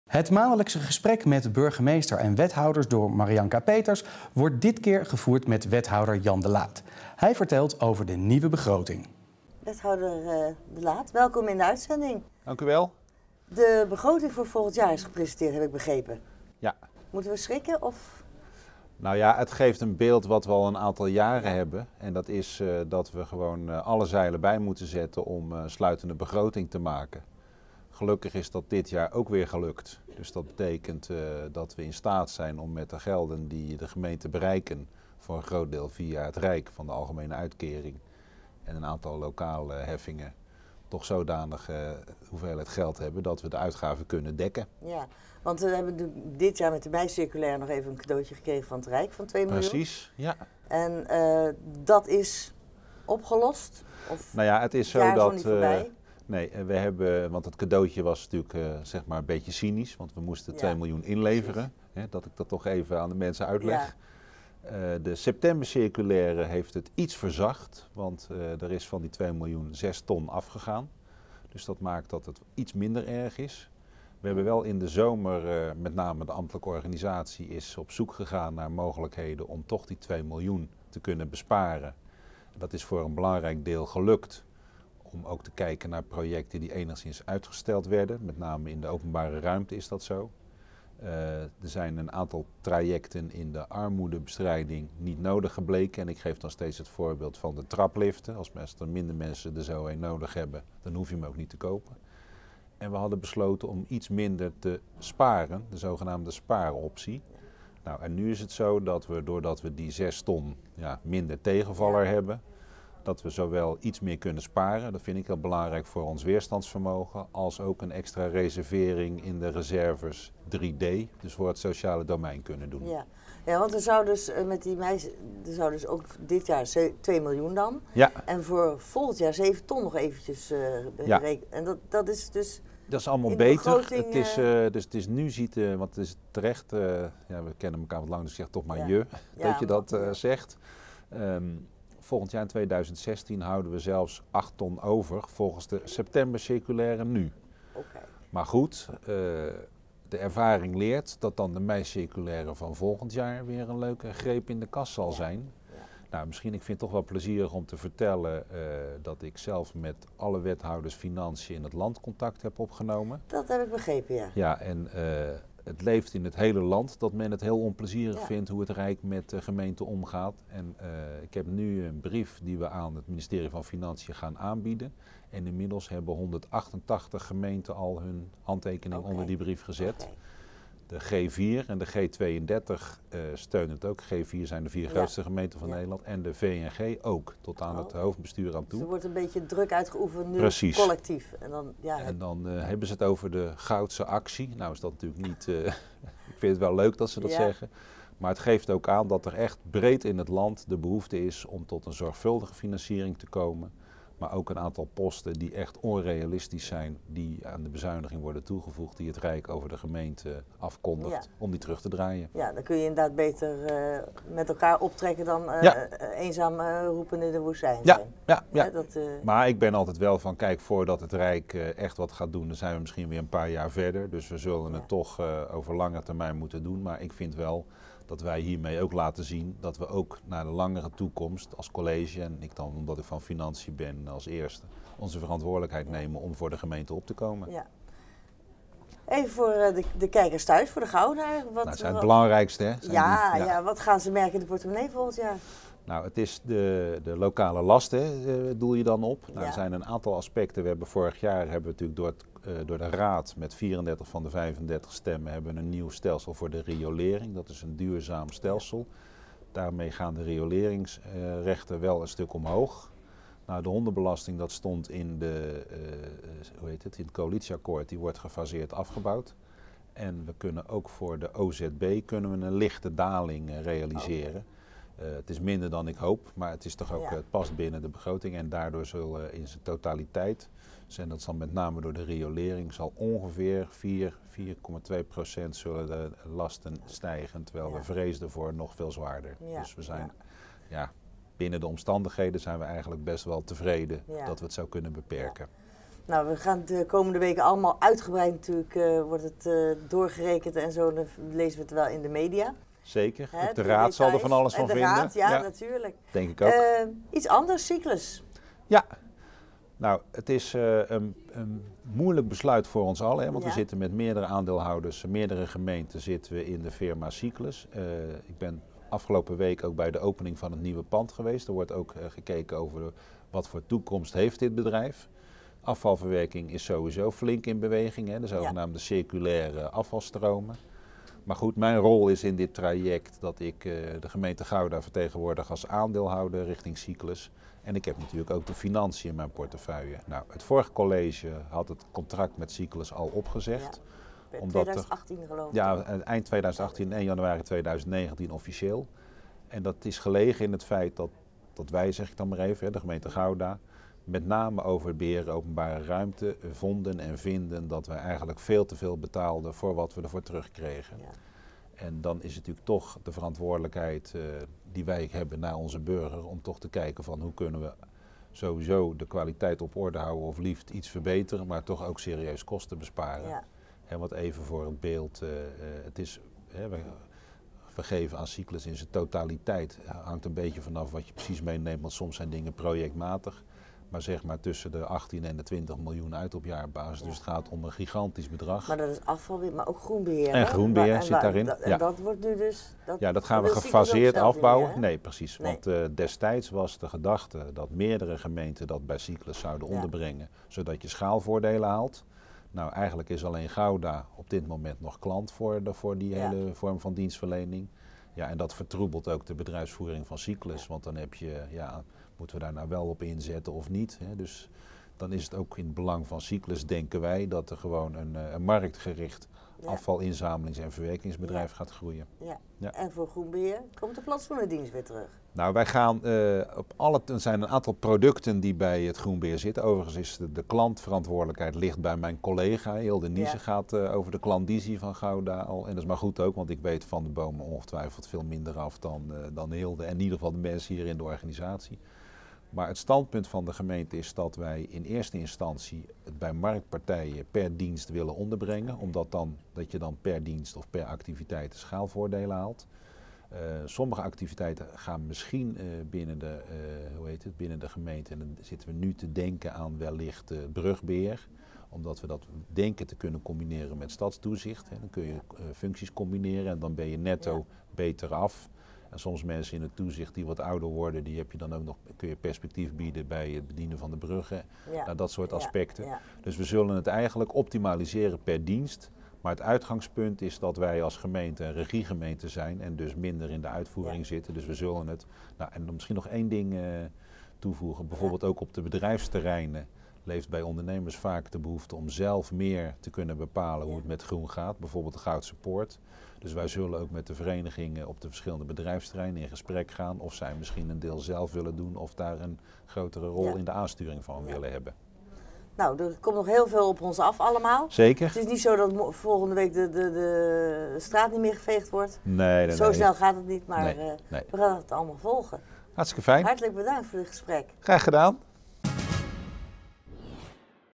2015 week 42 Wethouder Jan de Laat
Het maandelijkse gesprek met Burgemeester & Wethouders door Marianka Peters, wordt dit keer gevoerd met wethouder Jan de Laat.